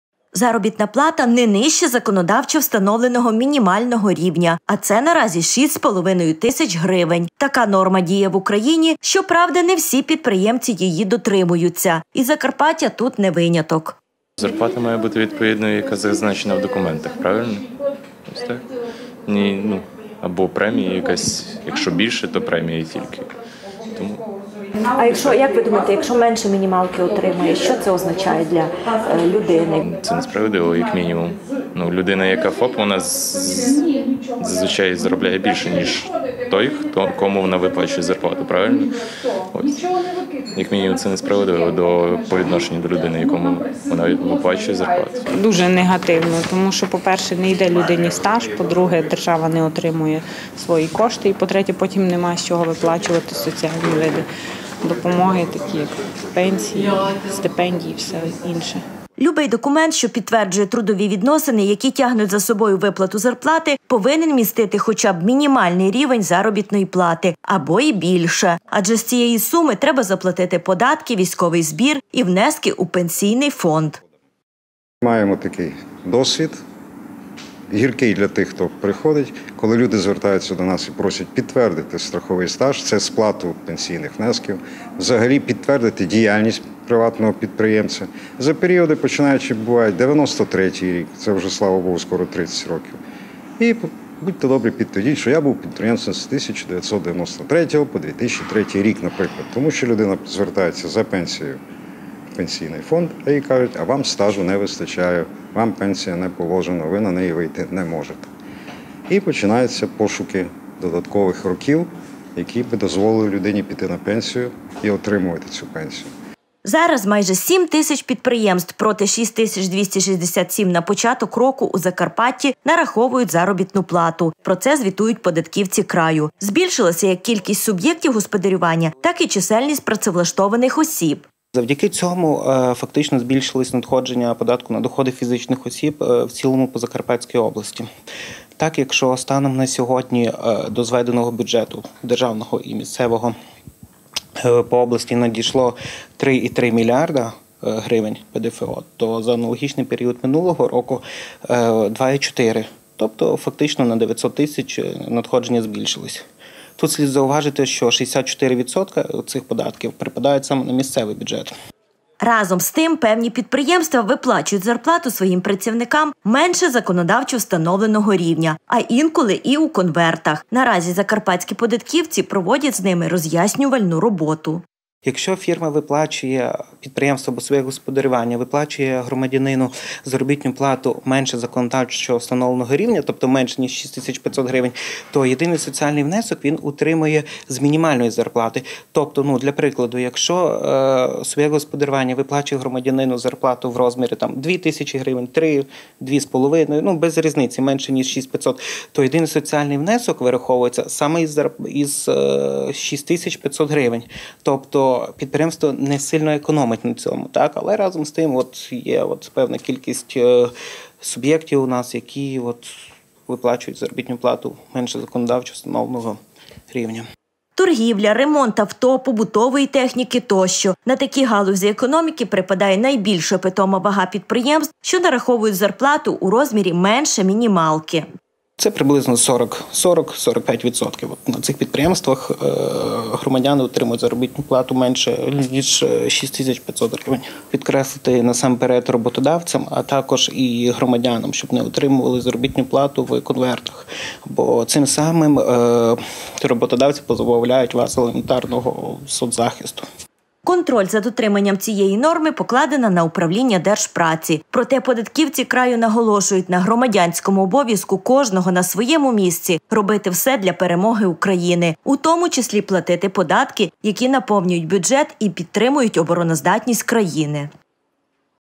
Не нижче мінімалки! Процедура нараховання заробітної плати і сплата податків
Працевлаштованих та суб’єктів господарювання у порівнянні з початком року у Закарпатті побільшало! Усі відповідно нараховують заробітну плату і сплачують податки. Завдяки цьому зросли і надходження податку на доходи фізичних осіб. Проте у краї досі залишаються юридичні особи, які виплачують своїм працівникам зарплату на рівні менше законодавчо встановленого рівня. Заробітна плата не нижче законодавчо встановленого мінімального рівня – а це наразі 6,5 тисяч гривень. Така норма діє в Україні, щоправда не всі підприємці  її дотримуються. І Закарпаття - тут не виняток. Любий документ, що підтверджує трудові відносини, які тягнуть за собою виплату зарплати, повинен містити хоча б мінімальний рівень заробітної плати або і більше! Адже з цієї суми треба заплатити податки, військовий збір і внески у пенсійний фонд. Зараз майже 7 тисяч підприємств, проти 6267 на початок року, у Закарпатті нараховують заробітну плату  - звітують податківці краю. Збільшилася як кількість  суб’єктів господарювання, так і чисельність працевлаштованих осіб. Разом з тим певні підприємства виплачують зарплату своїм працівникам менше законодавчо встановленого рівня. Наразі закарпатські податківці проводять з ними роз’яснювальну роботу.Торгівля, ремонт авто, побутової техніки тощо – на такі галузі економіки припадає найбільша питома вага підприємств, що нараховують зарплату у розмірі менше мінімалки! Контроль за дотриманням цієї норми  покладена на управління Держпраці. Проте податківці краю наголошують на громадянському обов’язку кожного на своєму місці робити все для Перемоги України, у тому числі платити податки, які наповнюють бюджеті і підтримують обороноздатність країни!